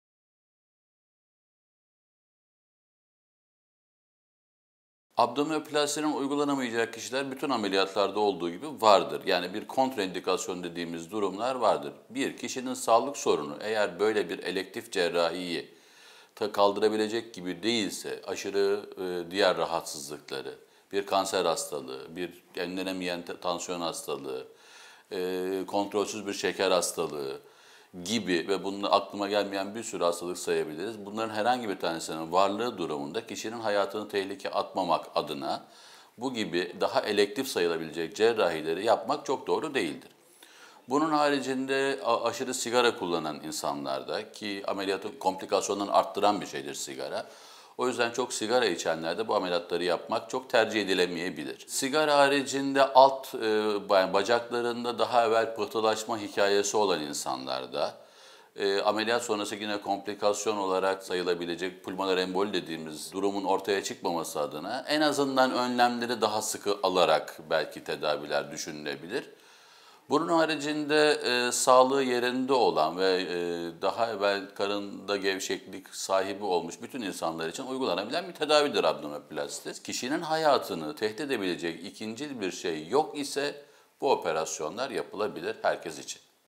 Karın Germe (Abdominoplasti ) uygulanamayacak kişiler var mıdır?
Acıbadem Kadıköy Hastanesi, Estetik, Plastik ve Rekonstrüktif Cerrahi Uzmanı
Prof. Dr. Berkhan Yılmaz Karın Germe (Abdominoplasti) üzerine soruları yanıtlıyor: